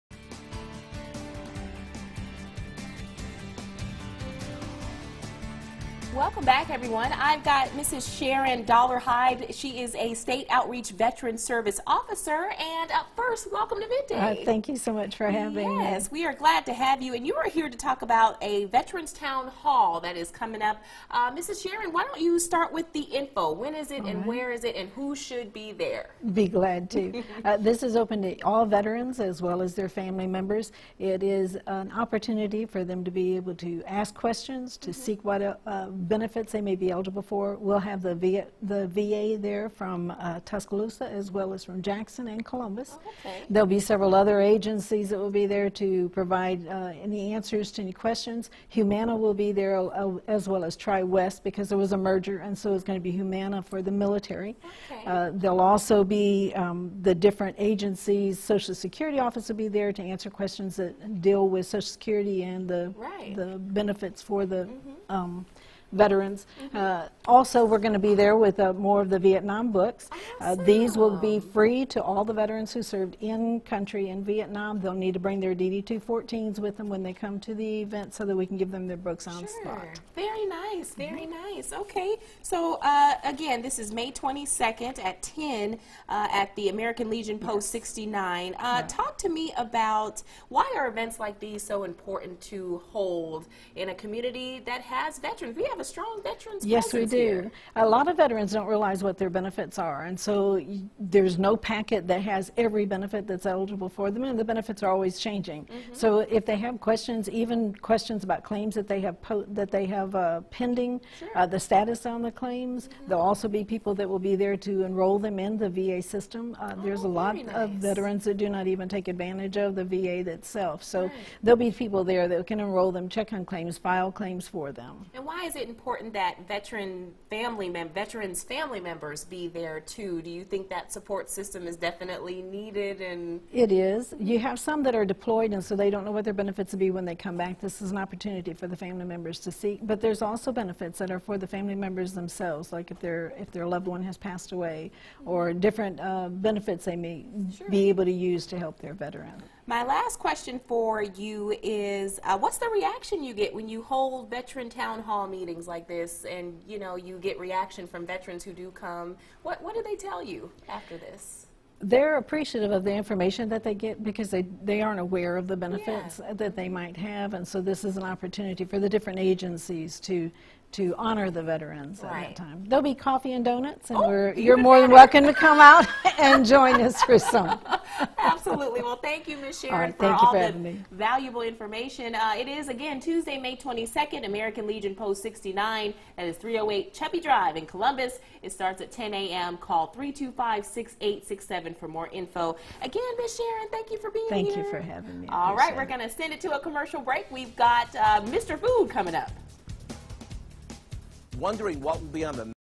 Midday Guest 5/17/18 - Veterans Town Hall
Sharon Dollarhide of the Mississippi Veterans Affairs Board will be hosting a Veterans Town Hall at American Legion Post 69 (308 Chubby Drive in Columbus) on Tuesday, May, 22nd to answer questions from veterans.  The meeting will begin at 10 AM and admission is free.